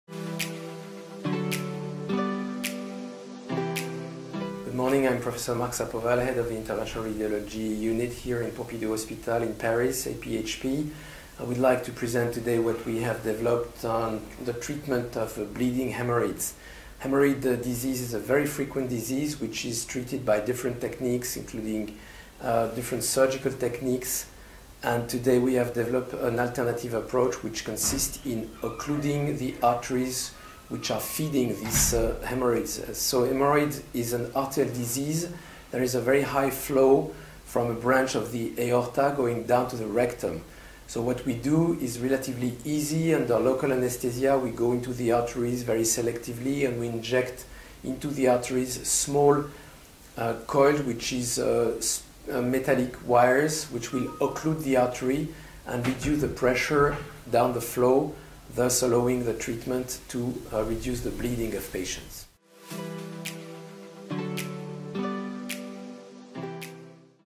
Hemorrhoids embolization
Pr Marc Sapoval Head of interventional radiology department presents the technic of hemorrhoids embolization.